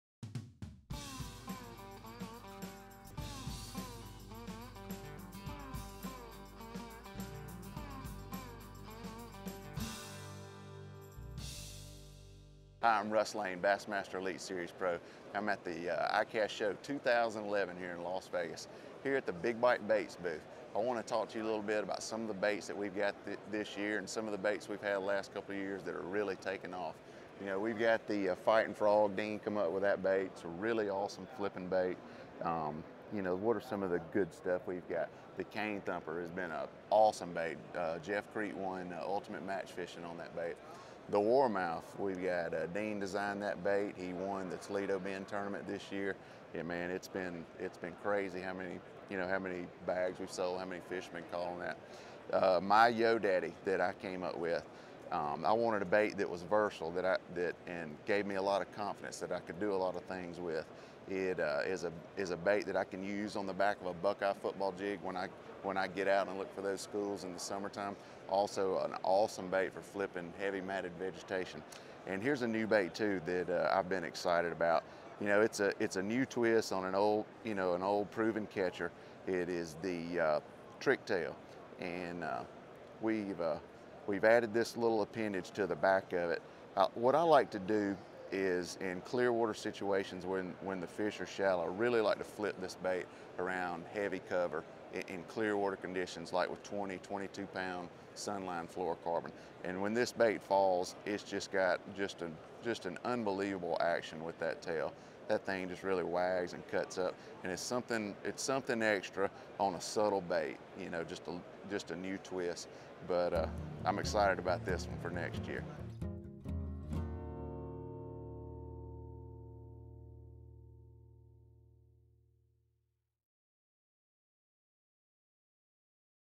Russ Lane from Big Bite Baits Booth at ICAST 2011
Touring Bass Angler Russ Lane discuss what's new with Big Bite Baits from ICAST 2011